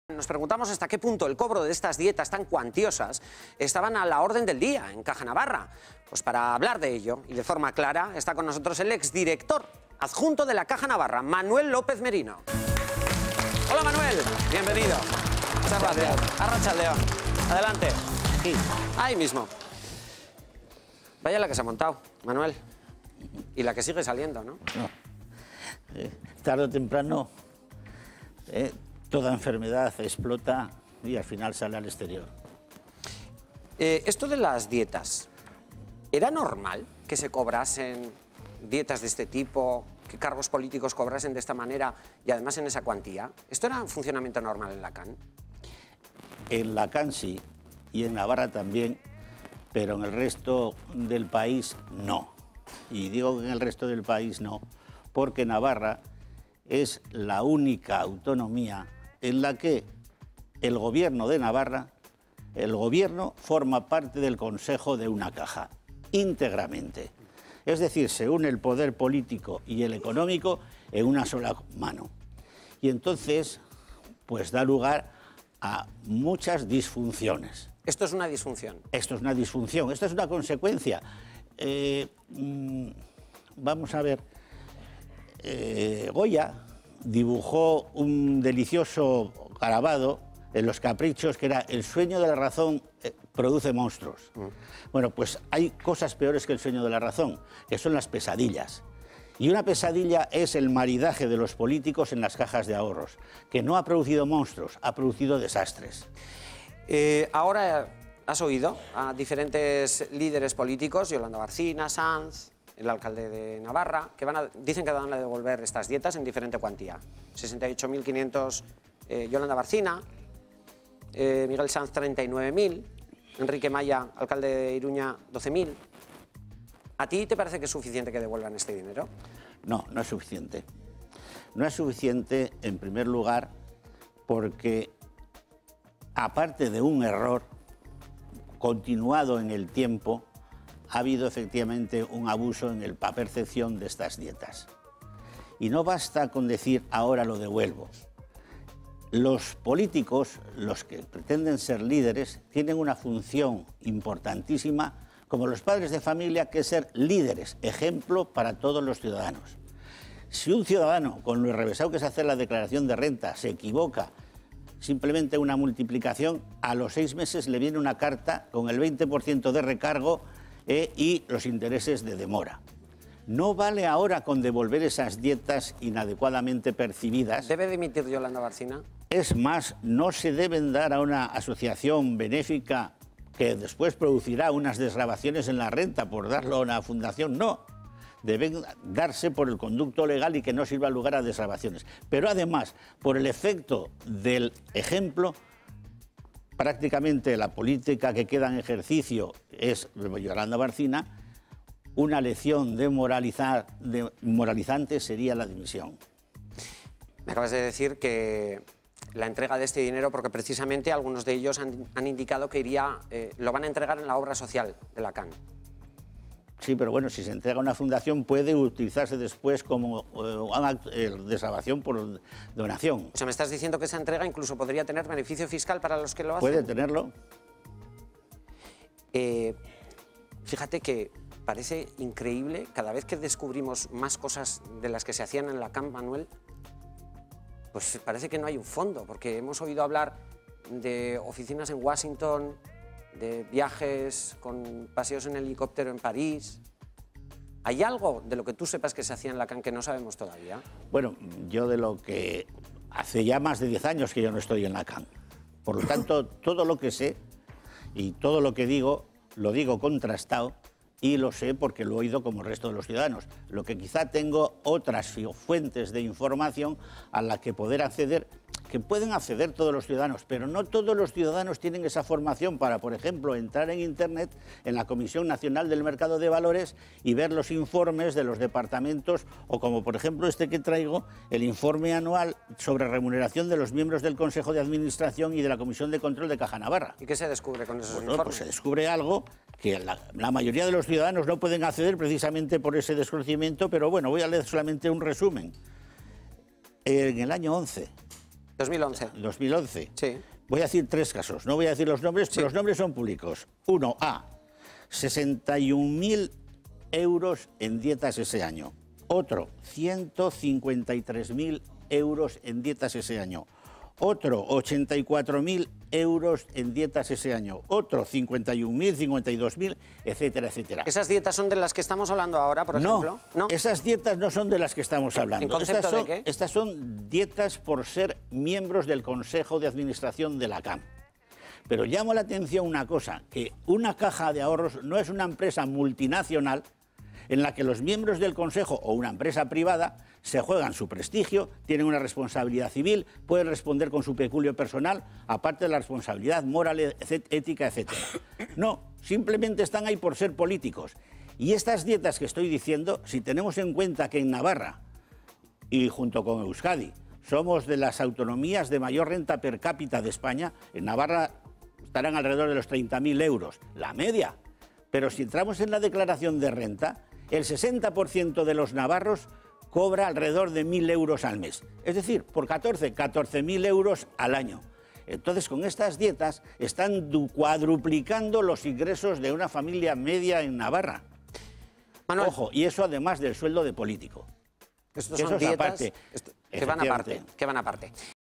Los sueldos por dietas cuadruplicaban un salario normal navarro
Sólo en dietas, los políticos cuadruplican los ingresos de una familia normal navarra. Según el ex director adjunto de la CAN, el funcionamiento de los cobros de dietas era común en Navarra.